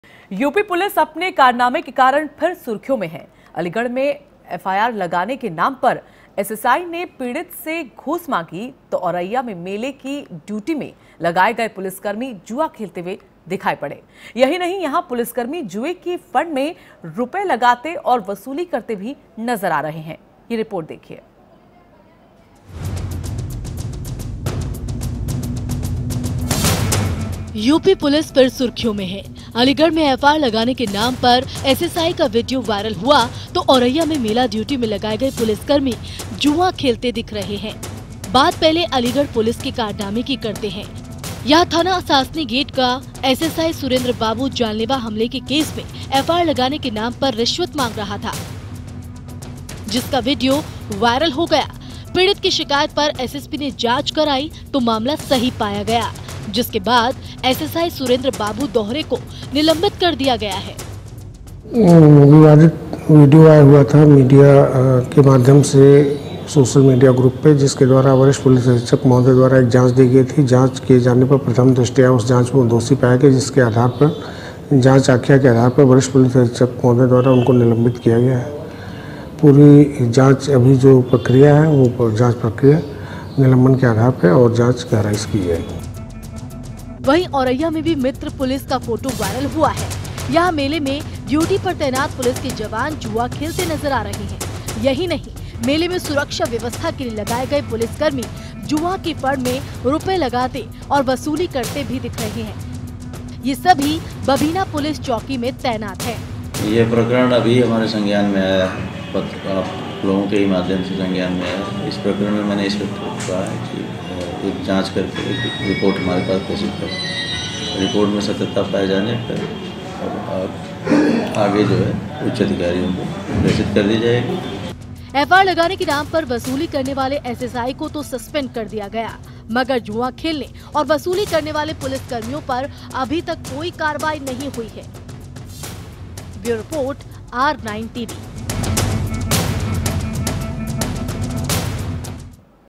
UP पुलिस अपने कारनामों के कारण फिर सुर्खियों में, देखिए रिपोर्ट
अलीगढ़ में FR लगाने के नाम पर SSI ने पीड़ित से घूस मांगी तो औरैया में मेले की ड्यूटी में लगाए गए पुलिसकर्मी जुआ खेलते दिखाई पड़ रहे हैं... देखिए रिपोर्ट